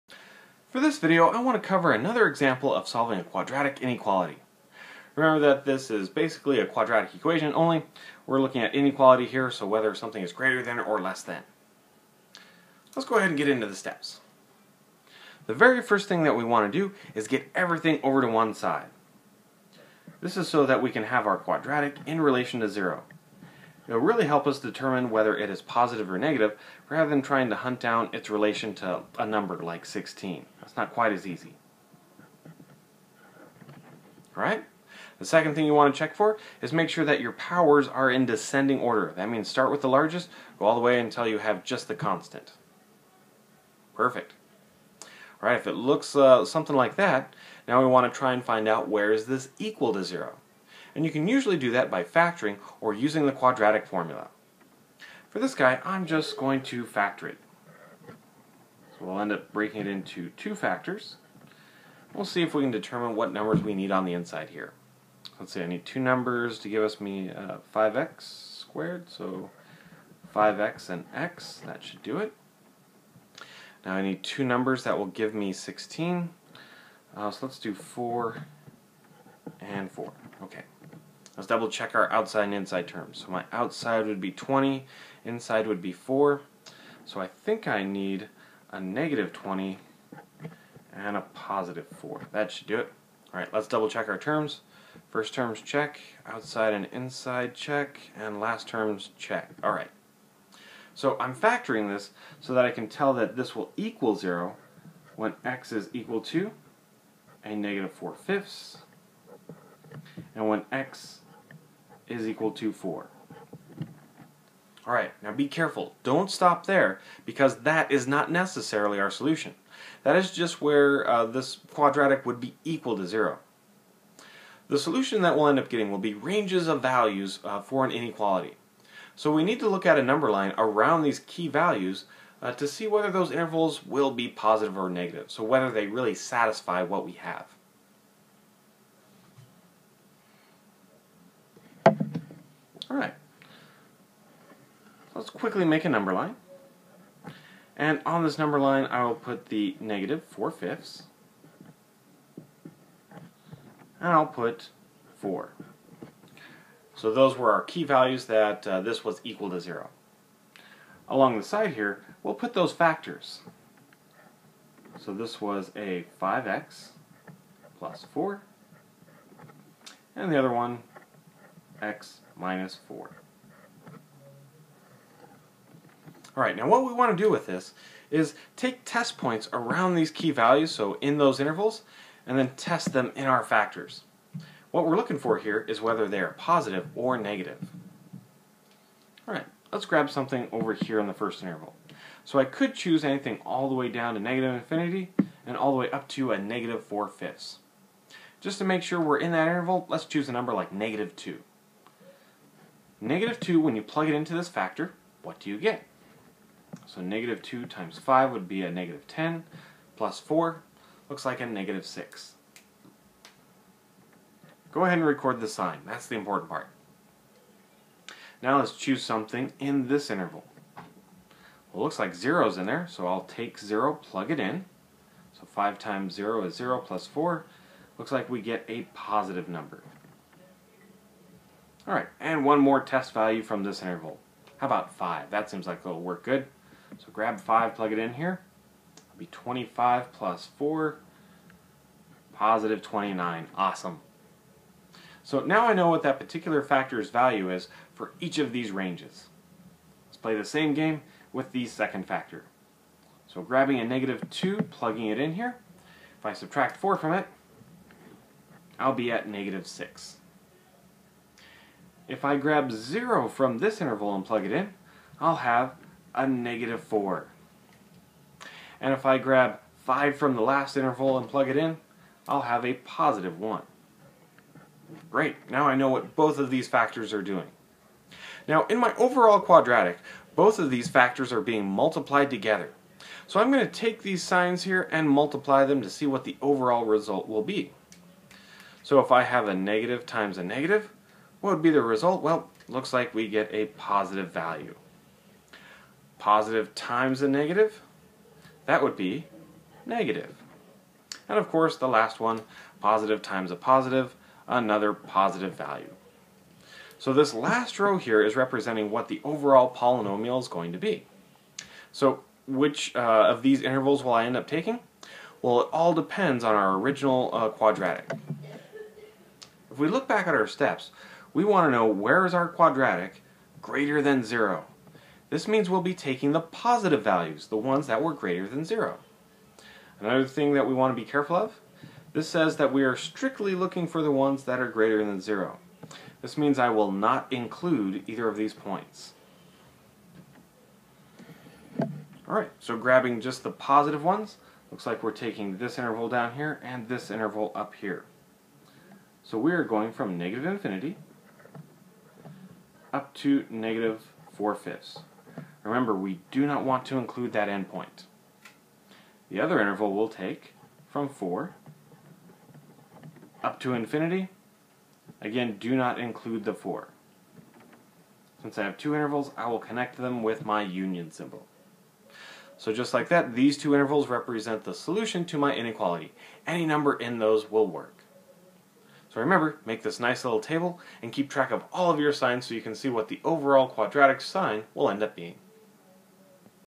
Pre-Calculus - Solving a quadratic inequality (2)
Here is another great example of solving a quadratic inequality.  Again a table is used to track down the proper intervals that satisfy the inequality.